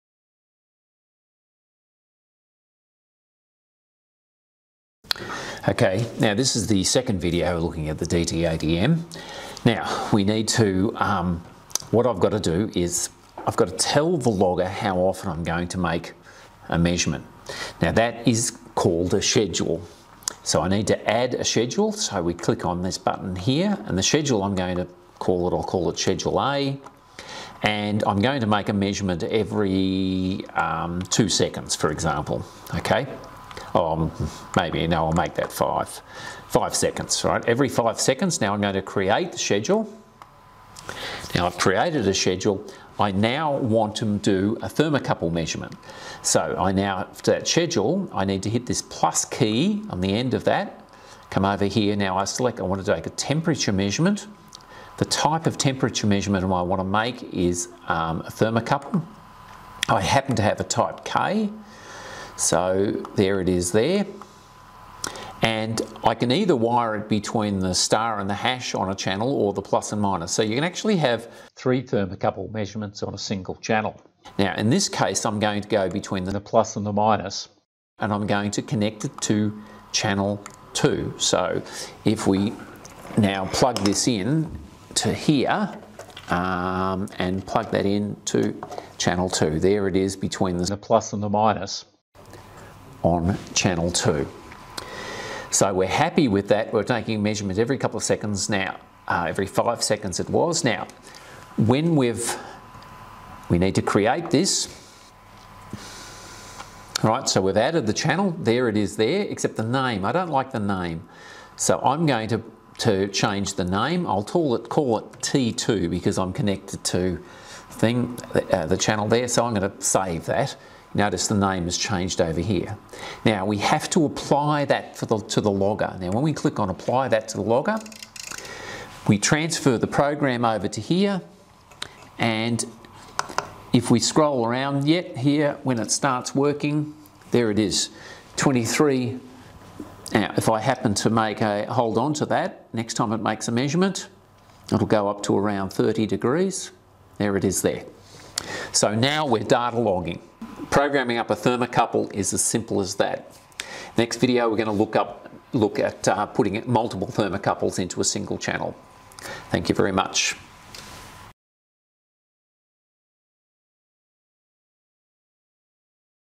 DataTaker DT80M - Video2: Thermocouple Measurement
This video demonstrates how to conduct a thermocouple measurement using the DT80M.